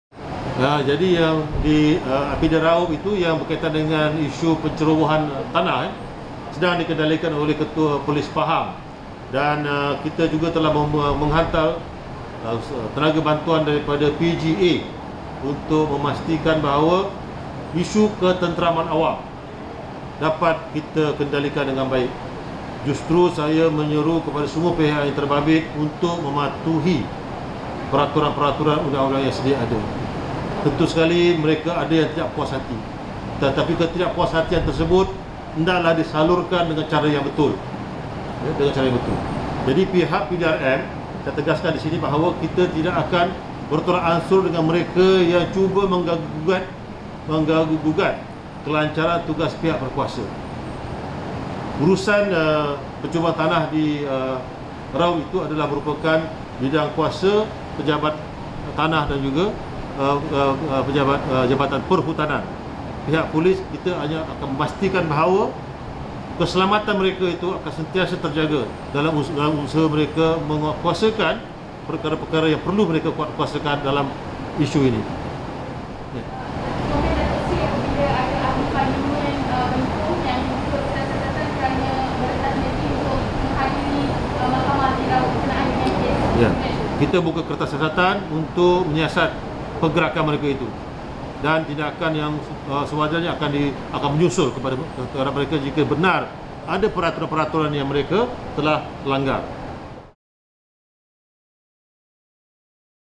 IGP: GOF deployed at Raub to ensure public order after recent issue during durian plantation ops
The General Operations Force (GOF) has been deployed to Raub, Pahang to ensure safety and public order following the recent issue involving durian farmers and local enforcement personnel there.

Inspector-General of Police Datuk Seri Acryl Sani Abdullah Sani said that on Tuesday (July 6), and urged those involved in the issue to adhere to the law, saying that police will not compromise in taking action against any party who disrupt the duties of the authorities.